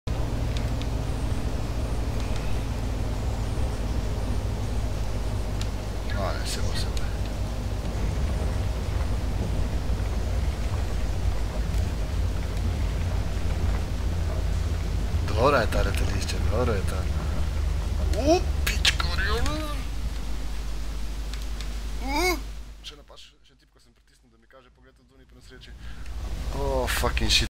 A bit too cocky when landing
This is what happens when you spot the airfield a bit too late and then still decide to land on it and then press camera change instead of brake. Now you know!

Airfield: Kangel Danda Airport (VNKL)
Airplane: Piaggio P.149